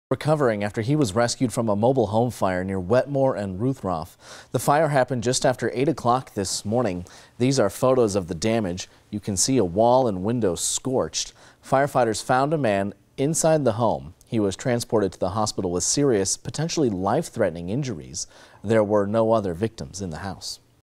Man taken to the hospital after house fire on northwest side
An adult man has been transported to the hospital with potentially life-threatening injuries after a mobile home fire on the northwest side Saturday, according to Capt. Brian Keeley, a spokesman for the Northwest Fire District.